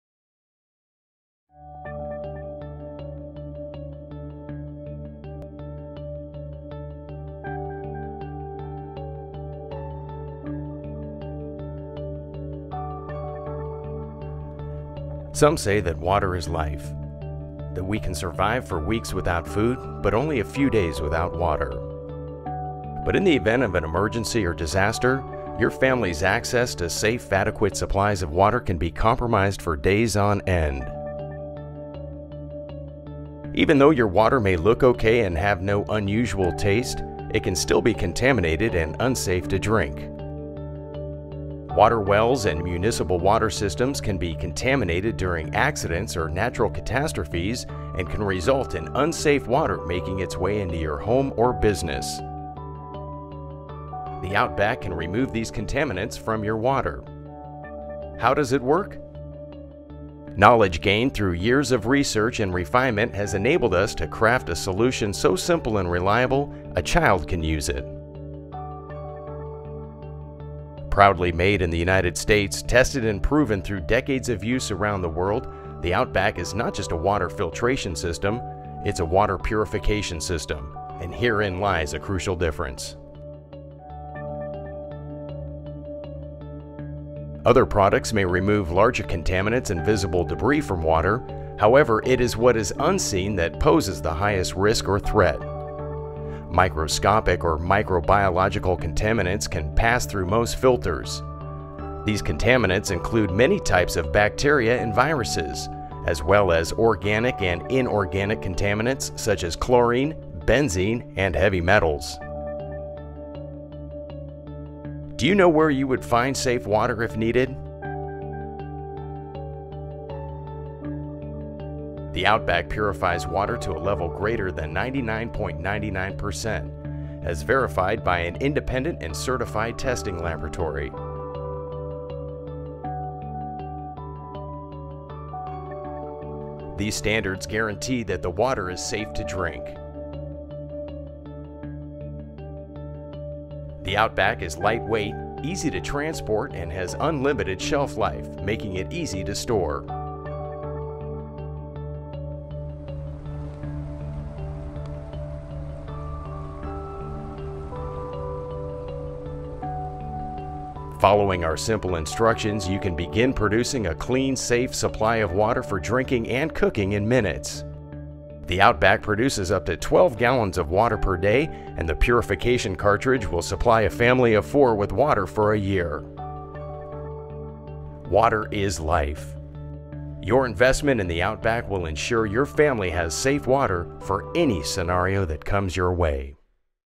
Outback Emergency Water Filter - Gravity Filtration System
The Outback removes bacteria at a rate of 99.9999% and viruses at 99.99%. Portable, no electricity required, and durable gravity feed filtration system. Excellent for daily use in emergency situations or in underdeveloped areas. Buckets nest for transport. Systems provide up to 1,800 gallons per day - enough water for a family of four for a year. Removes bacteria (such as those causing Cholera), cysts, and viruses (such as hepatitis, Sars and polio).